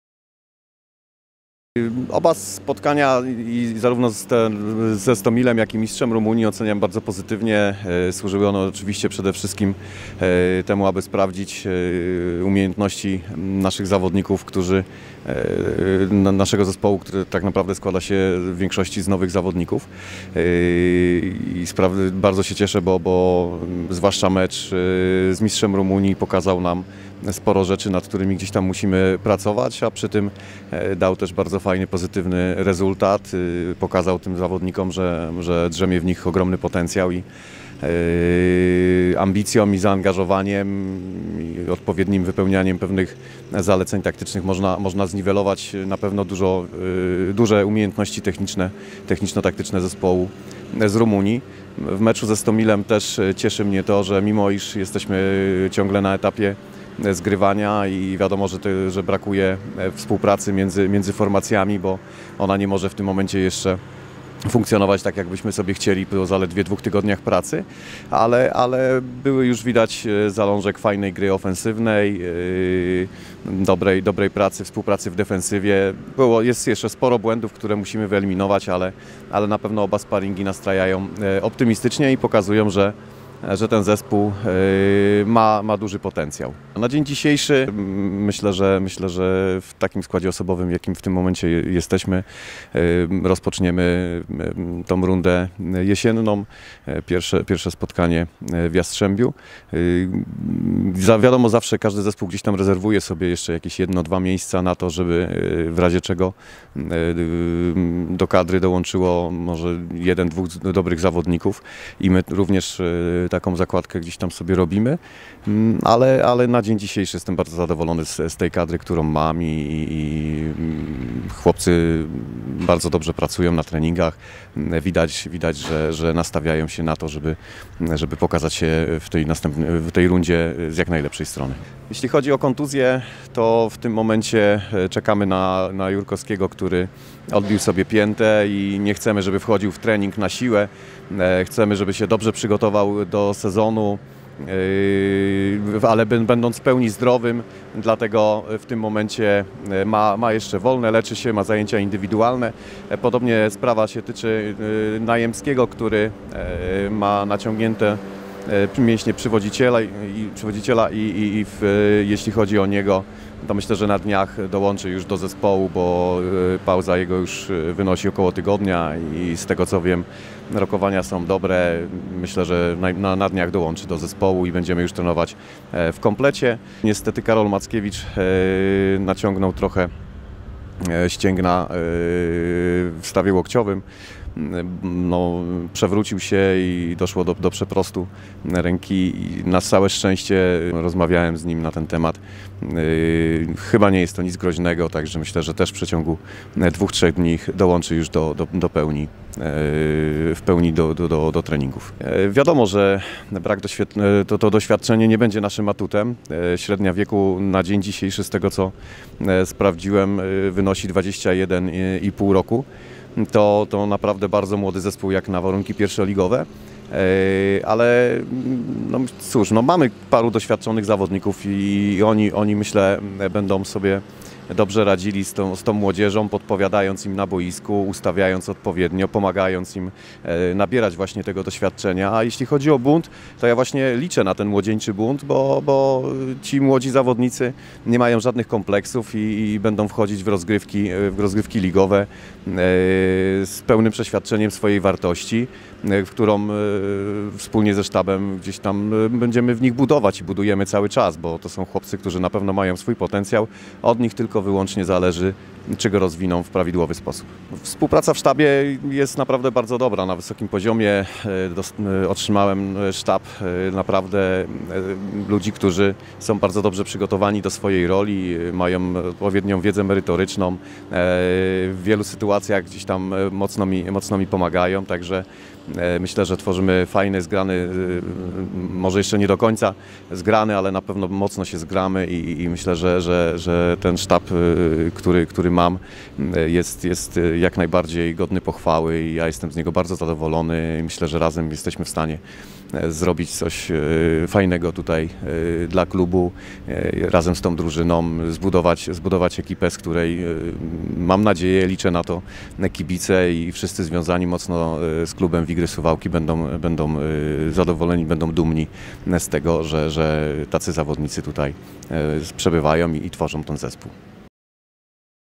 Socha: Oba sparingi oceniam pozytywnie
Zapraszamy na rozmowę z trenerem Wigier - Kamilem Sochą, który opowiedział nam o przebiegu okresu przygotowawczego oraz ocenił dwa ostatnie sparingi.